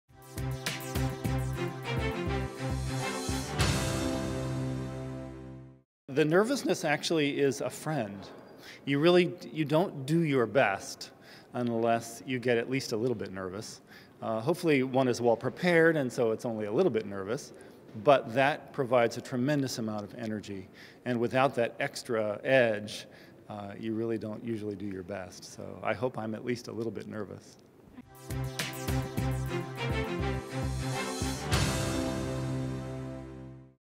Pianist Brian Ganz Uses Nervousness to Sharpen His Performances
Pianist Brian Ganz will be performing Feb. 7 and 8 at the Music Center at Strathmore. In this MyMCMedia extra video, Ganz comments on the role nervousness plays in his performances.